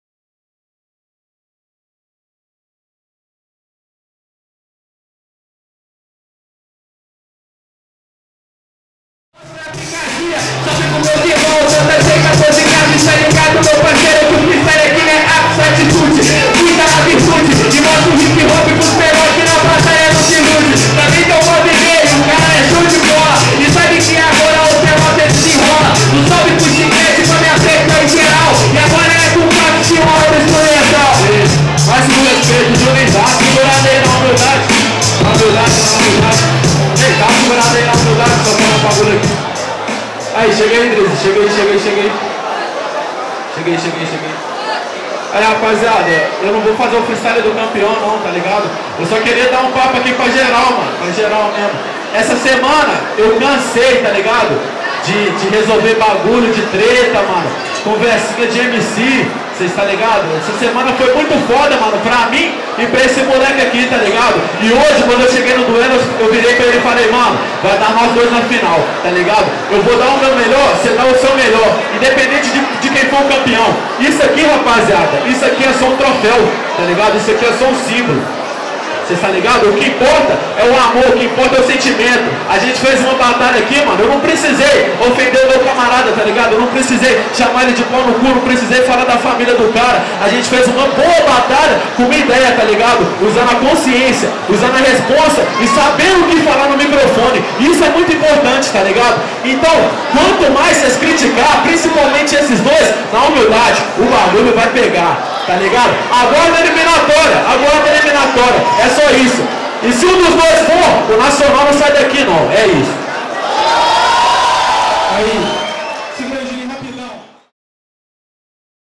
Freestyle do Campeão Drizzy - Seletivas MG Duelo de MCs Nacional - 08/10/17
VIDEOS - SELETIVAS MG DUELO DE MCS NACIONAL - 08/10/17

Dj Junin Bumbep

Imagens: Pâmela Bernardo